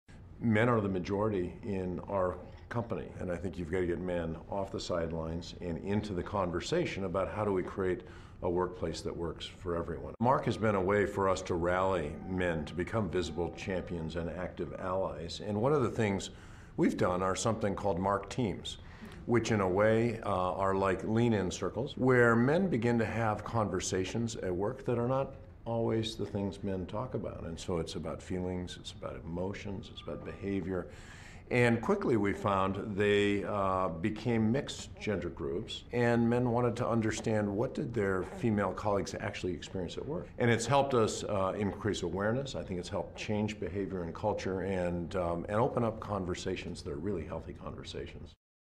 Men Advocating Real Change at Chevron: CEO Michael Wirth Speaks
Catalyst CEO & Chairman Michael Wirth on the impact Catalyst's MARC (Men Advocating Real Change) program has had on the workplace.